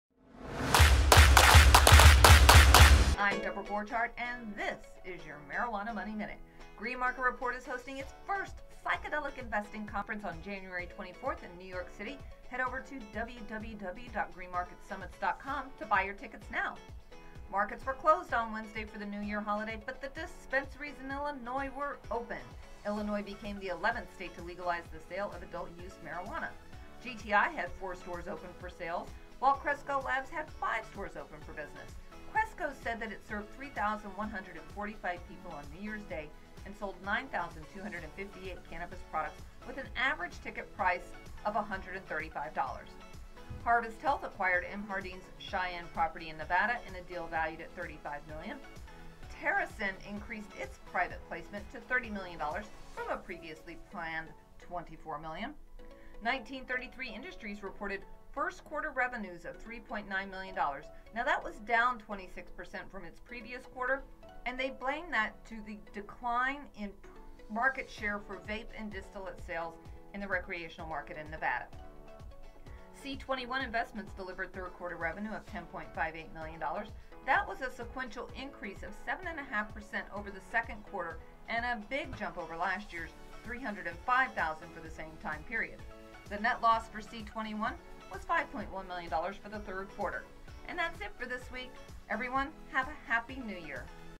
Green Market Report's Marijuana Money January 3, 2020
This is the Green Market Report's Marijuana Money for January 3, 2020. Stories include the start of adult-use cannabis sales in Illinois, earnings from 1933 Industries & C21 Investments, plus news about GMR's Psychedelic Investing Conference on January 24.